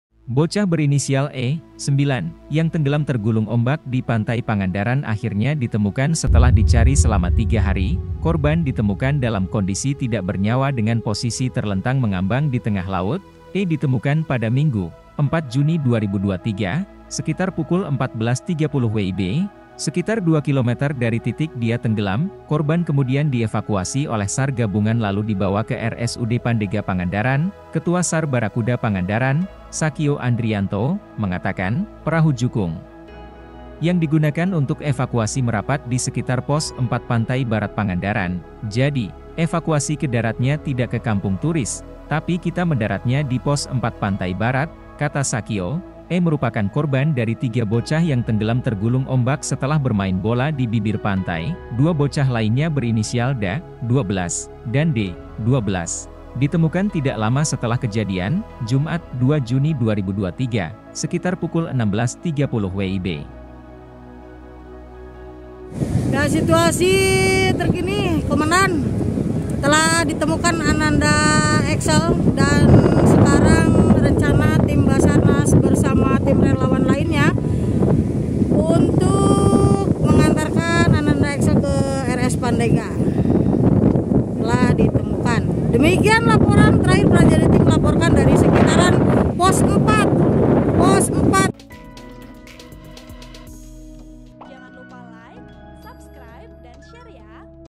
Bocah Tenggelam Digulung Ombak di Pangandaran Ditemukan di Tengah Laut, 2 Kilometer dari Titik Awal
Artikel ini telah tayang di TribunJabar.id dengan judul Bocah Tenggelam Digulung Ombak di Pangandaran Ditemukan di Tengah Laut, 2 Kilometer dari Titik Awal,
Penulis: Padna | Editor: Giri |Video Editor: Uben